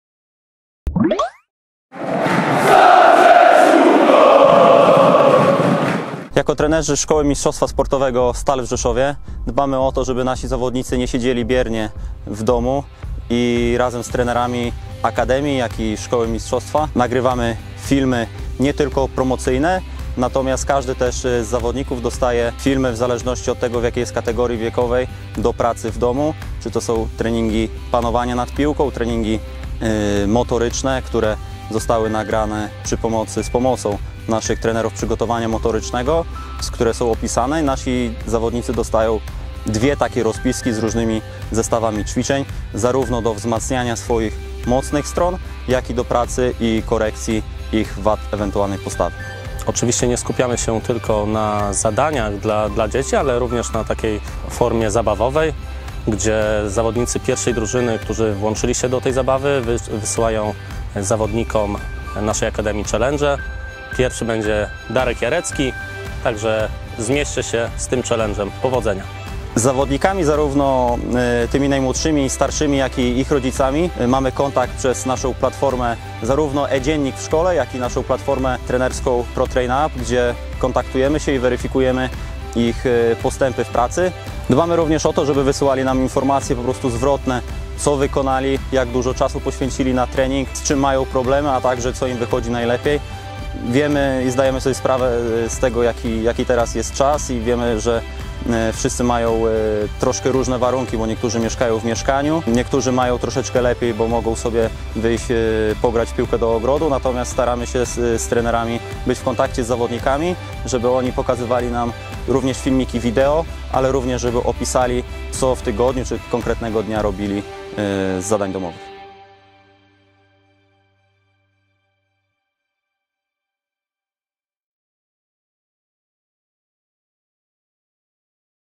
Praca trenerów podczas kwarantanny
Jeżeli chcecie się dowiedzieć jak wygląda praca trenerów podczas kwarantanny gdy nie spotykają się osobiście ze swoimi podopiecznymi.
Jak wyglądają treningi zawodników, jakie plany realizują i czy ktoś to weryfikuje.
Wszystkiego dowiecie się z poniższego filmiku  👇👇👇